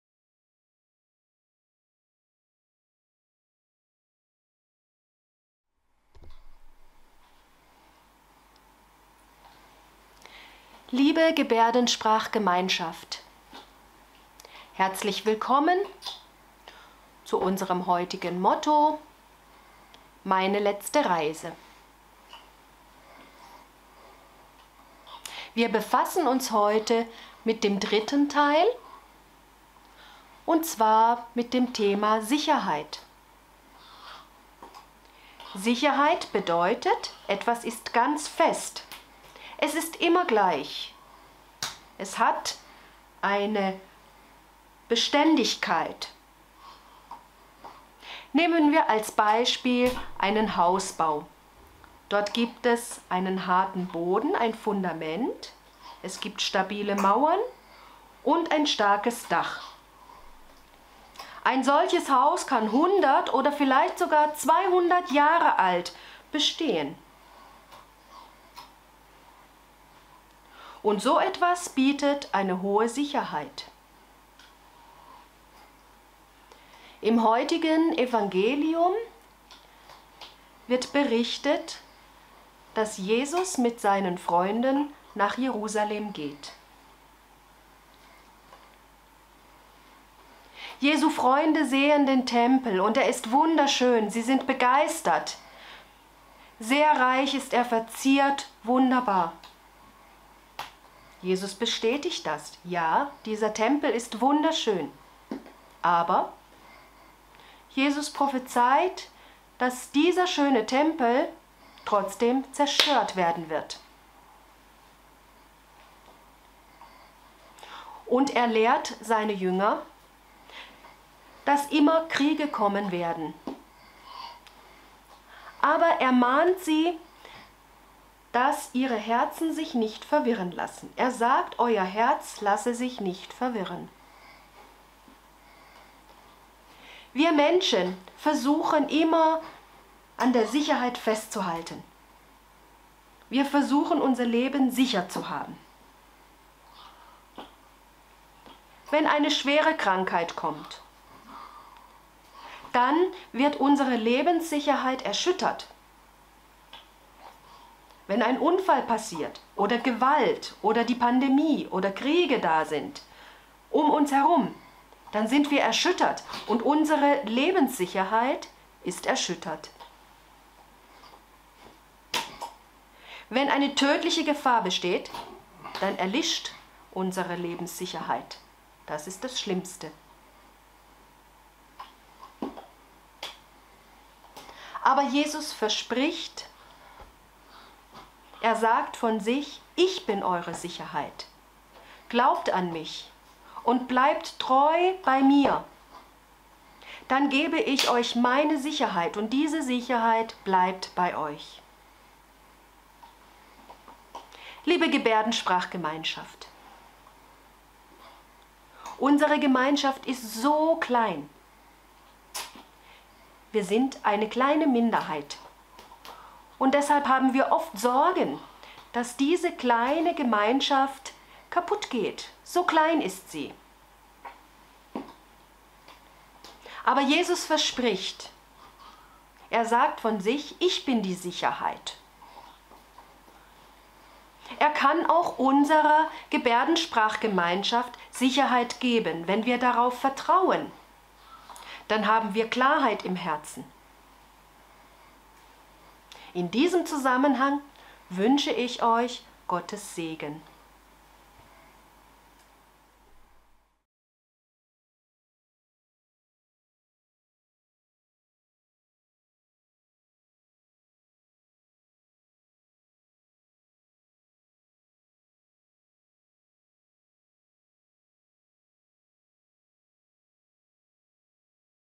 Mein Koffer für die letzte Reise - Sicherheit
TuK-Novemberaktion zum Thema Tod und Sterben. Eine Predigt von Diakon Peter Hepp mit dem Thema "Sicherheit" in Gebärdensprache und Lautsprache